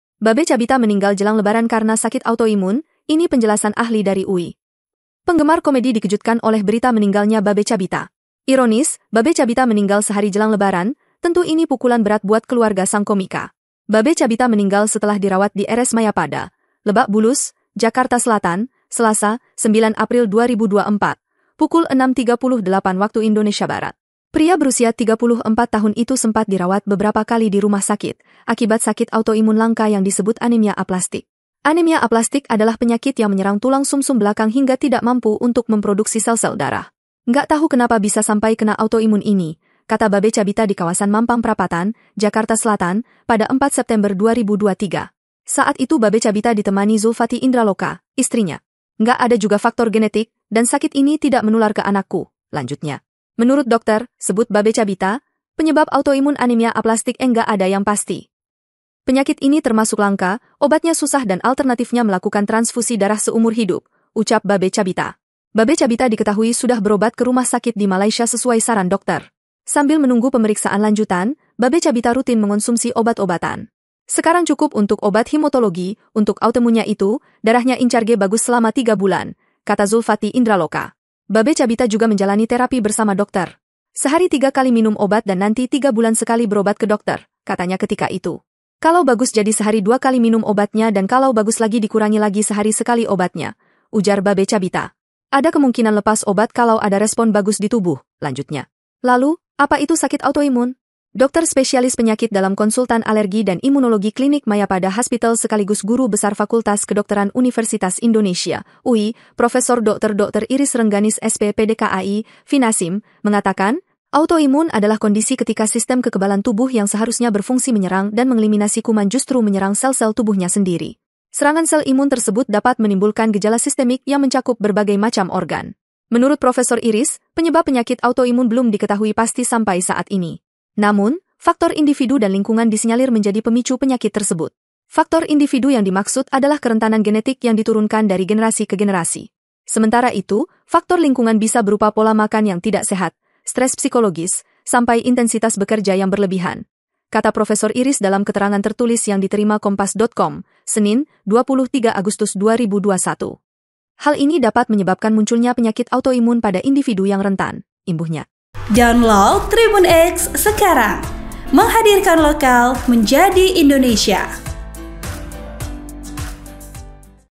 Babe Cabita Meninggal Jelang Lebaran Karena Sakit Autoimun, Ini Penjelasan Ahli dari UI
TRIBUN-VIDEO.COM - Penggemar komedi dikejutkan oleh berita meninggalnya Babe Cabita.

Ironis, Babe Cabita meninggal sehari jelang lebaran, tentu ini pukulan berat buat keluarga sang komika.

Babe Cabita meninggal setelah dirawat di RS Mayapada, Lebak Bulus, Jakarta Selatan, Selasa (9/4/2024), pukul 06.38 WIB.

Pria berusia 34 tahun itu sempat dirawat beberapa kali di rumah sakit, akibat sakit autoimun langka yang disebut Anemia Aplastik.

TribunTernate.com
VP : Ega Awellana